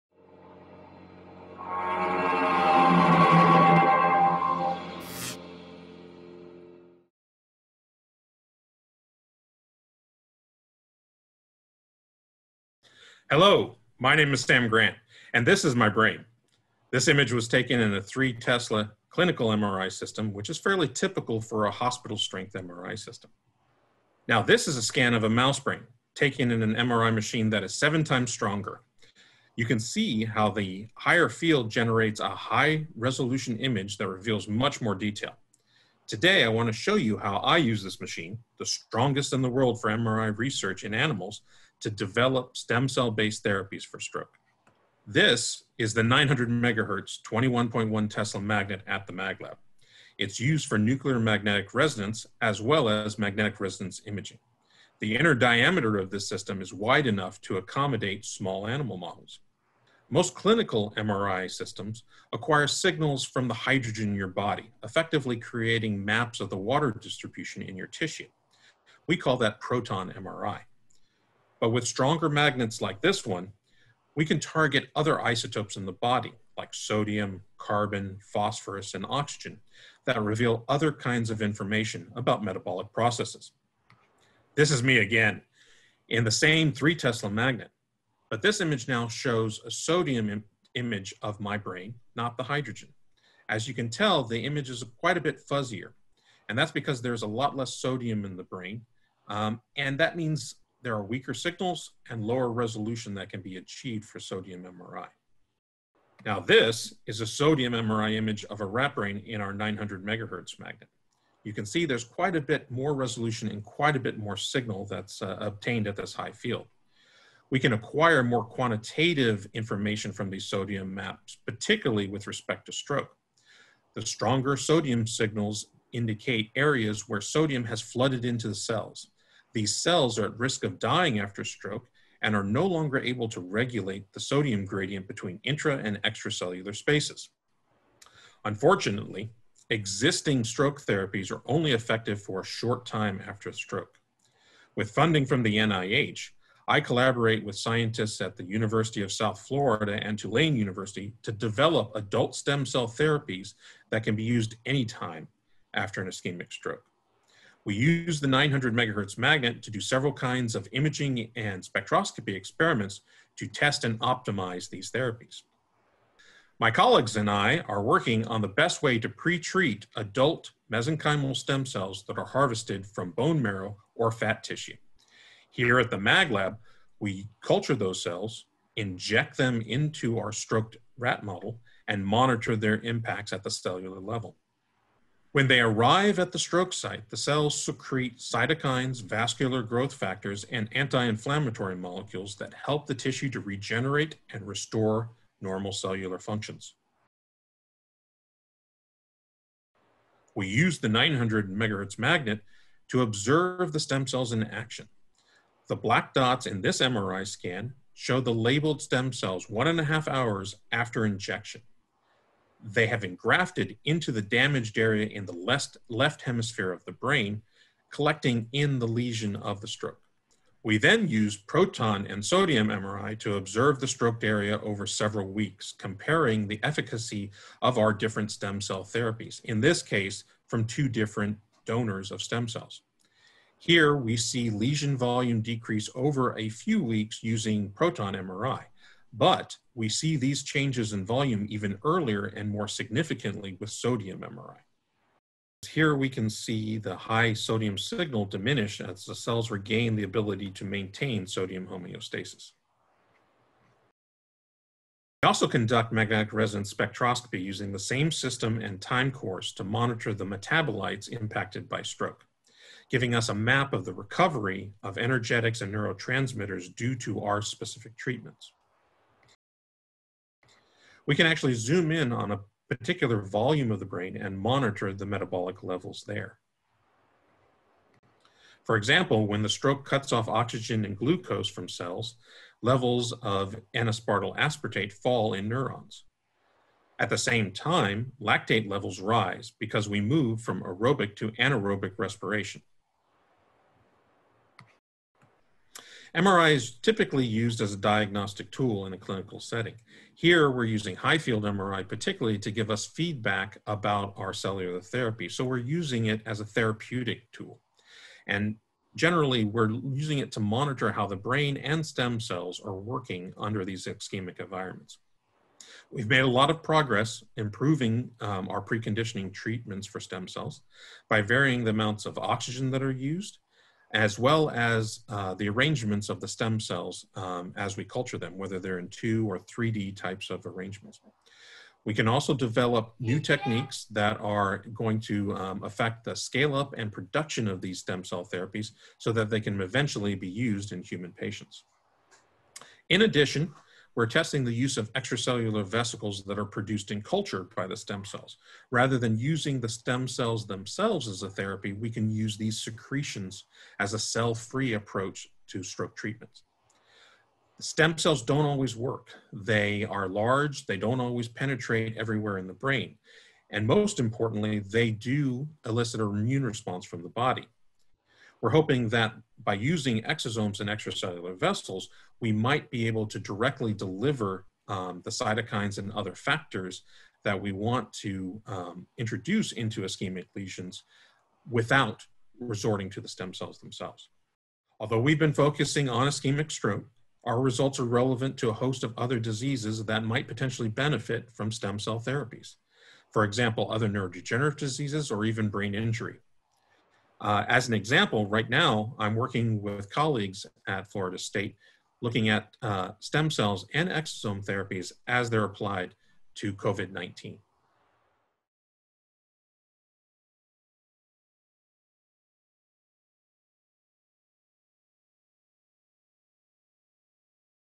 Salt, Stem Cells and a New Way to Treat Stroke
Featuring National MagLab MRI Program Director Dr. Sam Grant